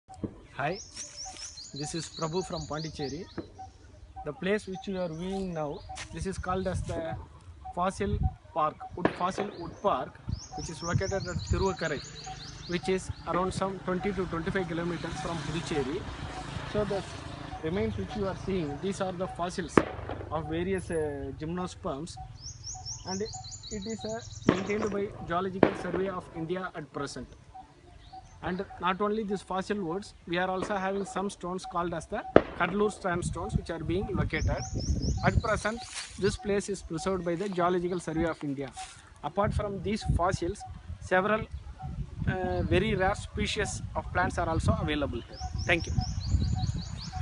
National Fossil Wood Park, Tiruvakkarai,VILLUPURAM DISTRICT
A SHORT VIDEO CLIP ON National Fossil Wood Park, LOCATED AT Tiruvakkarai, VILLUPURAM DISTRICT,TAMILNADU.

The park contains petrified wood fossils approximately 20 million years old, scattered throughout the park, which covers about 247 acres (100 ha). The park consists of nine enclaves, but only a small portion of the 247 acres (approx 1 square km) is open to the public. Officials of the GSI believe the fossils were formed during massive flooding that occurred millions of years ago.

The park hosts about 200 fossilized trees. They range in size from 3 to 15 metres (9.8 to 49.2 ft) meters in length, some of which are up to 5 meters in width. They are strewn and partially buried in the park grounds. No branches or leaves remain on the fossilized trunks.

Scientists speculate that the trees did not originally grow at the site, but were transported before they had petrified. M. Sonneret, a European naturalist, gave the first detailed account of the fossils in 1781.
The fossils are well preserved due to extensive petrifaction. The trees' annular rings and pit structures are clearly visible, allowing their age to be determined by counting the rings